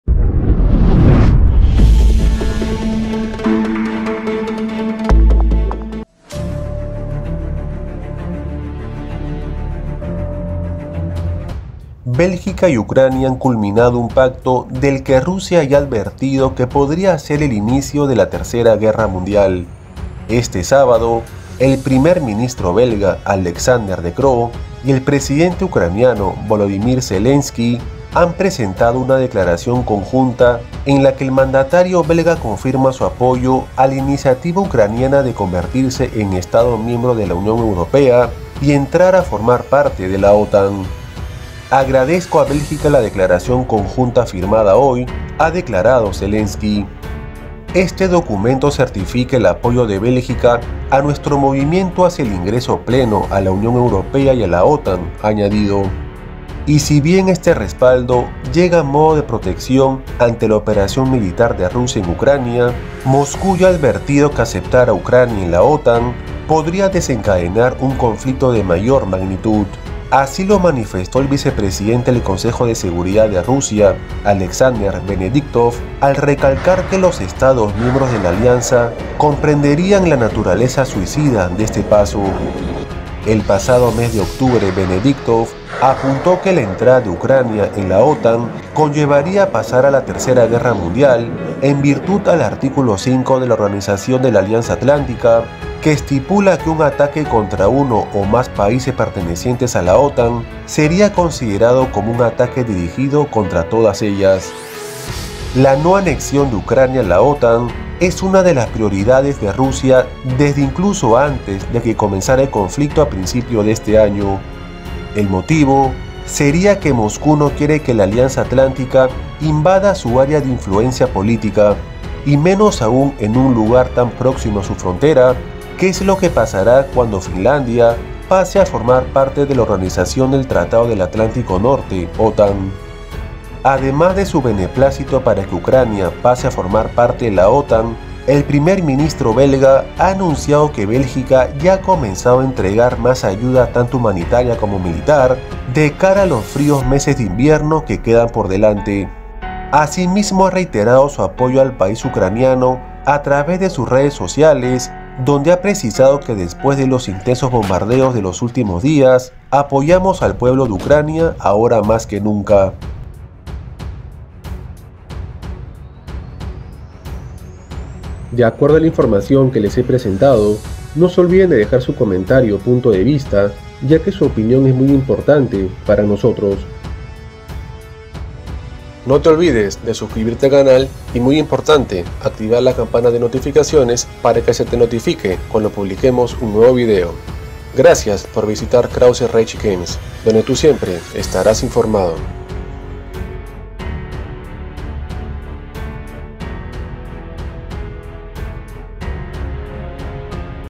🔴LO ÚLTIMO : UCRANIA y Bélgica Firman Pacto de Apoyo MILITAR ante seria Advertencia de RUSIA
Esta Información viene desde Ucrania , esperamos sea de su interés. Buen día.